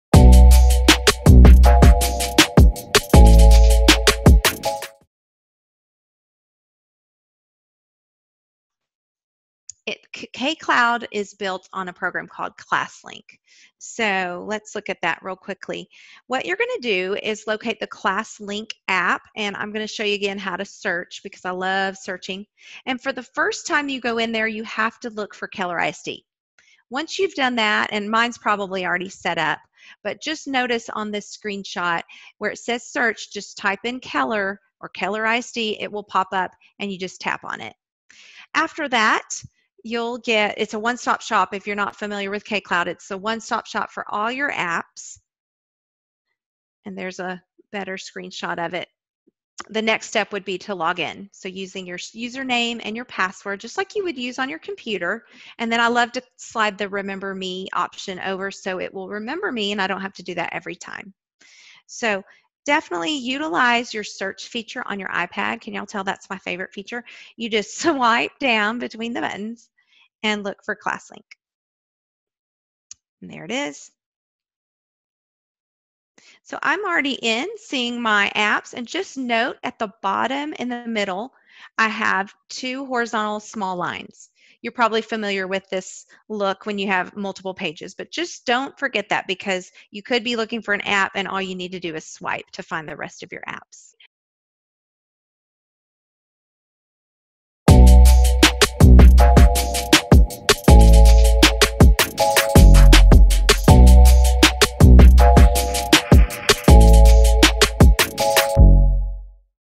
08 iPad Basics Series - KCloud Classlink
This series of short videos will help you familiarize yourself with the iPad environment (known as iOS) from EdTechKISD.